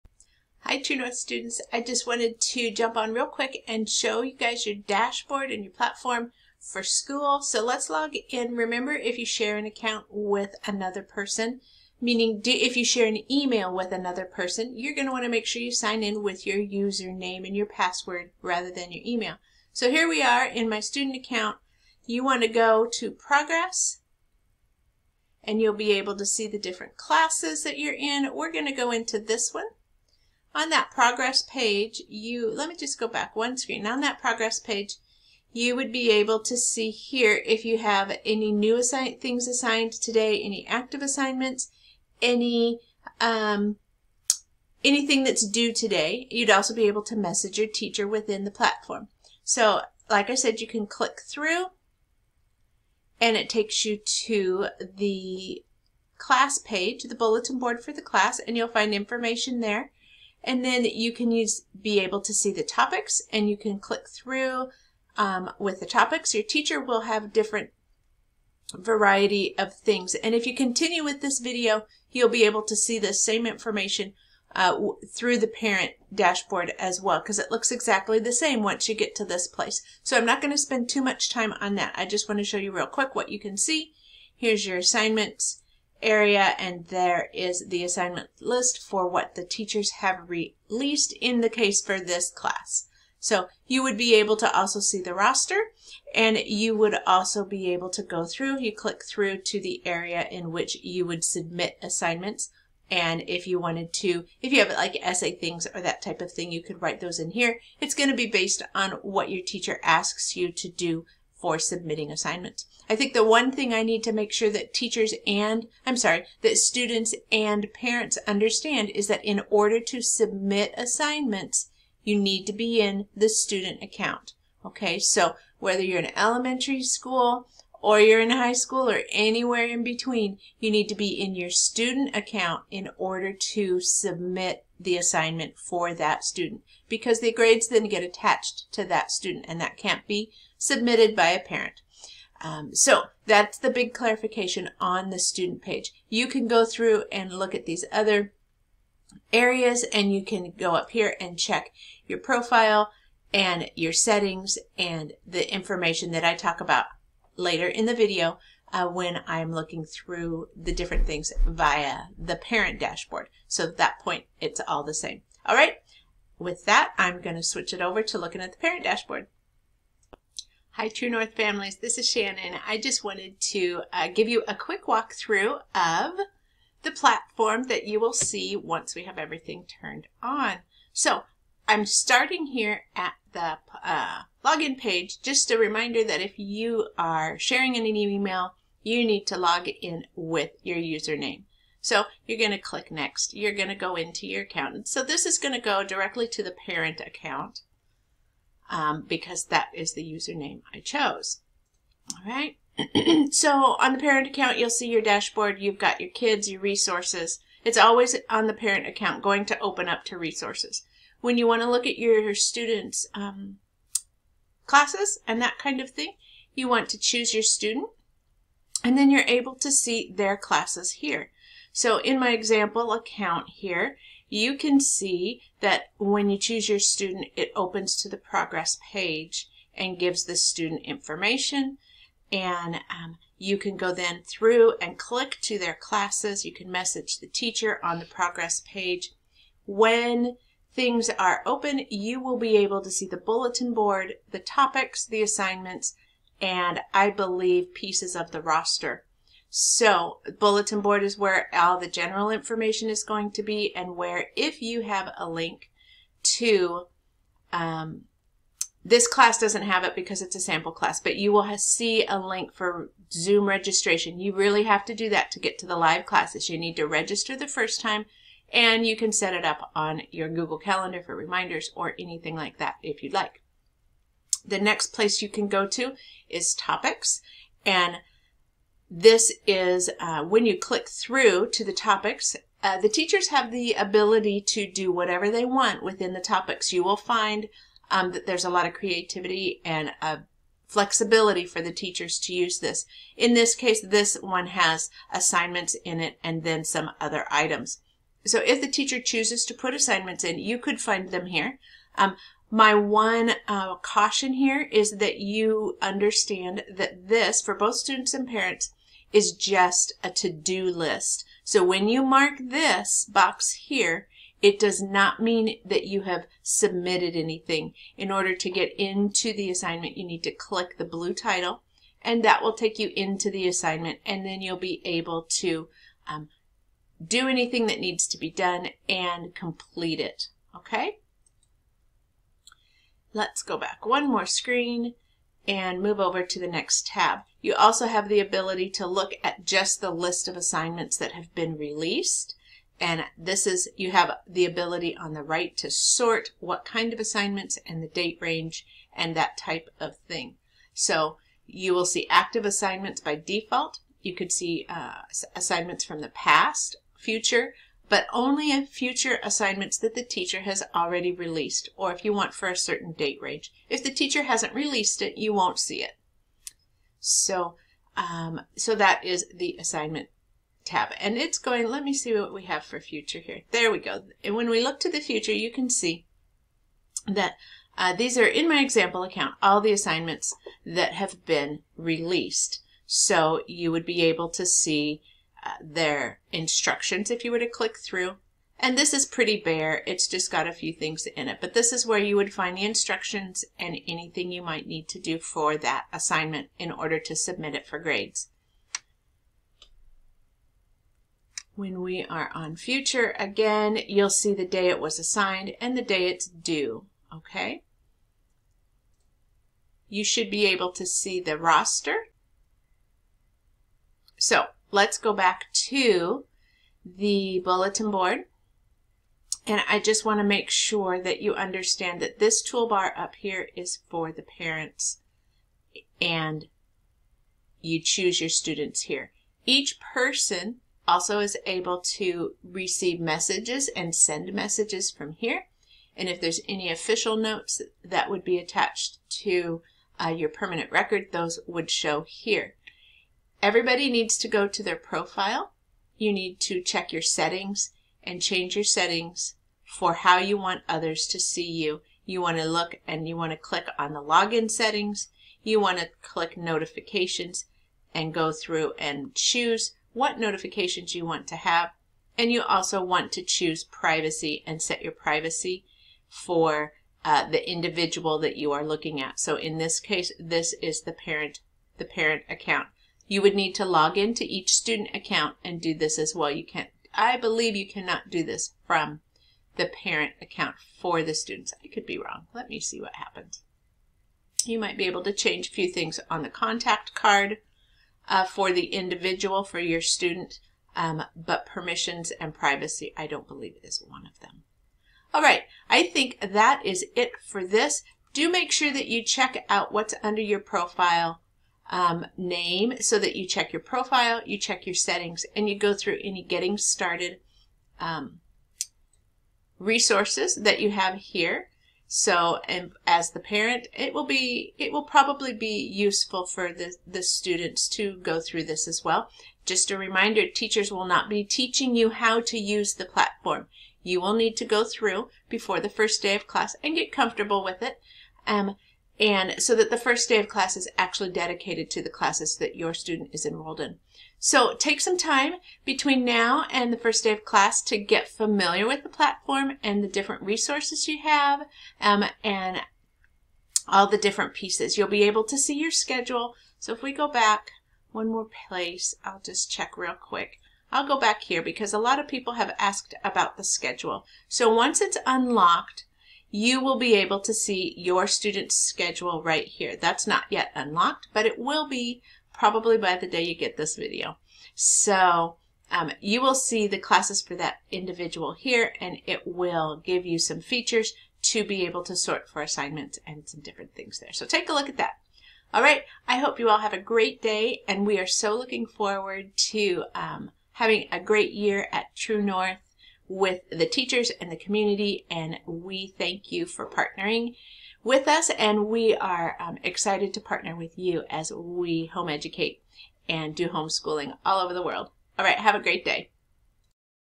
MySchool App Parent & Student Dashboard Overview
For all enrolled True North Homeschool Academy Parents and Students. This is a quick overview of your Parent and Student Dashboards.